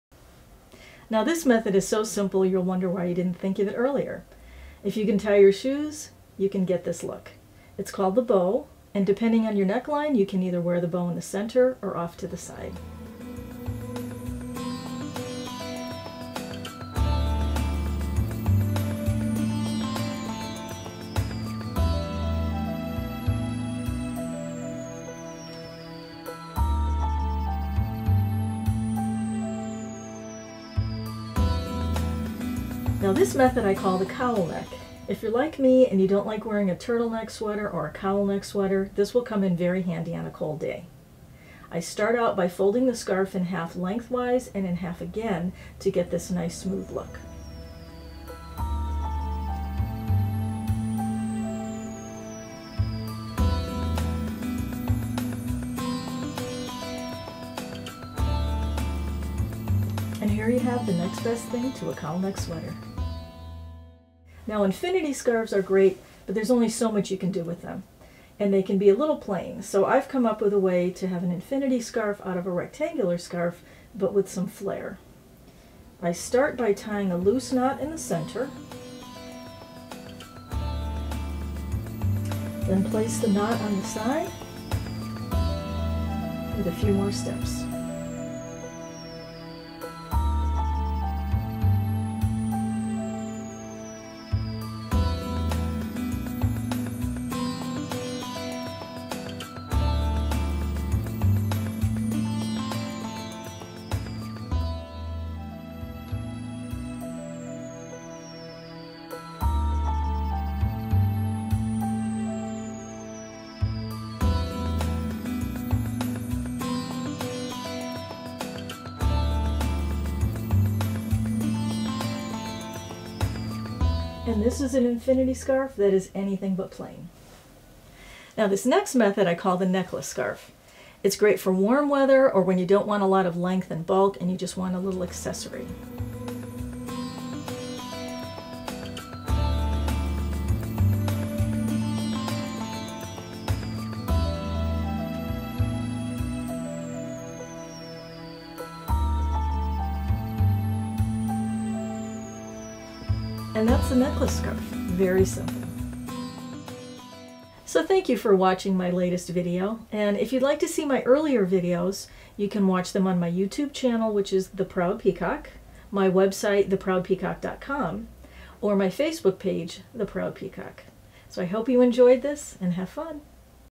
Scarf How To: The Bow; The Cowl Neck; Infinity Scarf; Necklace Scarf by Proud Peacock
4 Different Scarf Tying Methods by The Proud Peacock, Kathie Plaskiewicz- The Bow, The Cowl Neck, Infinity Scarf, Necklace Scarf.